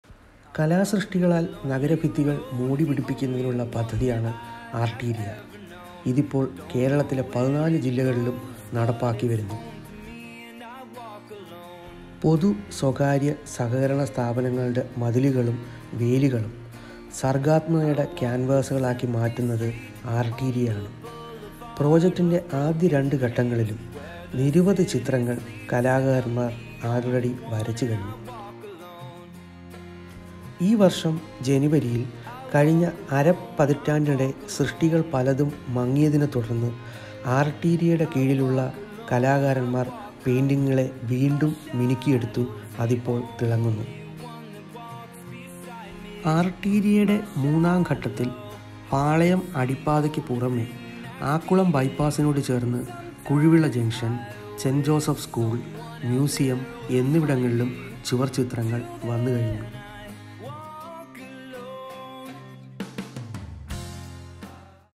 Arteria | to Lend Life to City Wall | Trivandrum | Imaging
‘Arteria’ to Lend Life to City Walls

More colours were splashed this year to embrace the festive mood of Onam in the city of Thiruvananthapuram, as project Arteria, a Kerala Tourism initiative turned the city walls into a canvas for renowned artists.

“The concept of turning city walls into canvases for artistic expressions mainly aims at the beautification of the city and to spread awareness among the public about contemporary art and paintings,” said Mr. A. P. Anilkumar, Minister for Tourism, Kerala.

He also noted that attracting the public eye to art works of this dimension facilitates their participation in a new artistic culture that assumes greater significance, considering the fact that ‘our public’s exposure to contemporary art is limited.’

Arteria gained momentum when the walls of University Stadium got painted with colourful images of artistic eloquence. A team of 20 artists, including renowned Kanayi Kunhiraman, Kattoor Naryana Pillai, B. D. Dathan, N. N. Rimzon and Tensing Joseph participated in the first phase of this art project. Other than the involvement of established artists, Arteria also provided scope for aspiring artistes and students as well.

“The plan to invite artworks from lesser known artists and art students is an effort to bring promising talents to the limelight and let the world know about their talents,” said Mr. G. Kamala Vardhana Rao, Secretary, Kerala Tourism.